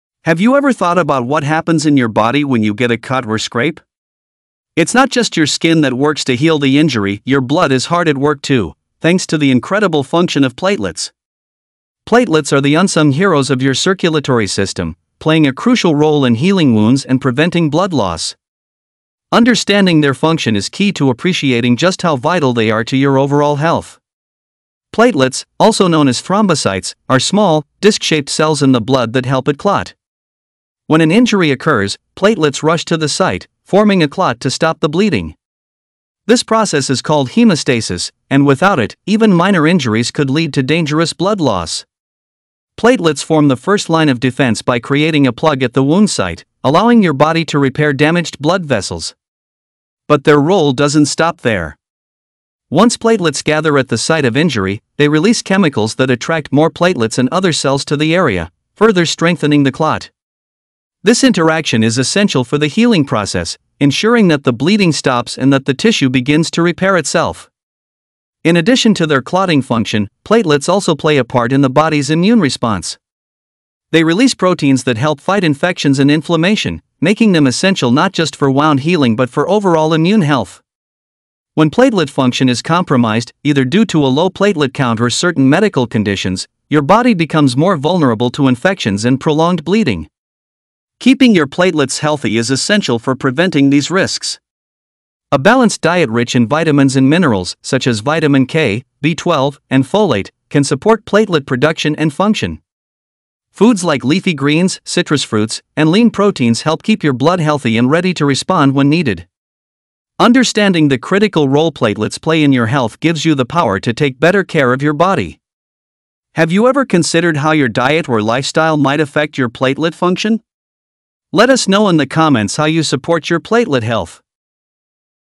The Vital Function of Platelets and Why You Should Care About Them
Have you ever thought about what happens in your body when you get a cut or scrape? It’s not just your skin that works to heal the injury—your blood is hard at work too, thanks to the incredible function of platelets. Platelets are the unsung heroes of your circulatory system, playing a crucial role in healing wounds and preventing blood loss. Understanding their function is key to appreciating just how vital they are to your overall health.
Platelets, also known as thrombocytes, are small, disk-shaped cells in the blood that help it clot. When an injury occurs, platelets rush to the site, forming a clot to stop the bleeding. This process is called hemostasis, and without it, even minor injuries could lead to dangerous blood loss. Platelets form the first line of defense by creating a plug at the wound site, allowing your body to repair damaged blood vessels.
But their role doesn’t stop there. Once platelets gather at the site of injury, they release chemicals that attract more platelets and other cells to the area, further strengthening the clot. This interaction is essential for the healing process, ensuring that the bleeding stops and that the tissue begins to repair itself.
In addition to their clotting function, platelets also play a part in the body’s immune response. They release proteins that help fight infections and inflammation, making them essential not just for wound healing but for overall immune health. When platelet function is compromised, either due to a low platelet count or certain medical conditions, your body becomes more vulnerable to infections and prolonged bleeding.
Keeping your platelets healthy is essential for preventing these risks. A balanced diet rich in vitamins and minerals, such as vitamin K, B12, and folate, can support platelet production and function. Foods like leafy greens, citrus fruits, and lean proteins help keep your blood healthy and ready to respond when needed.
Understanding the critical role platelets play in your health gives you the power to take better care of your body. Have you ever considered how your diet or lifestyle might affect your platelet function?